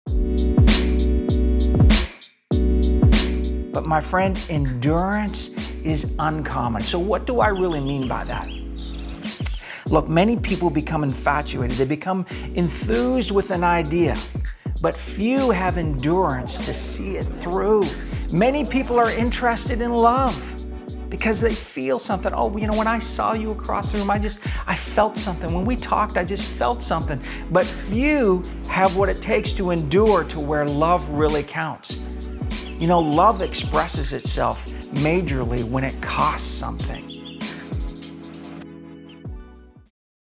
ENDURANCE IS UNCOMMON // Pastor Stephen Marshall
You need endurance to see the victory!

We are here to cover, value & encourage you to LIVE LIFE STRONG! For inspiration and answers go to: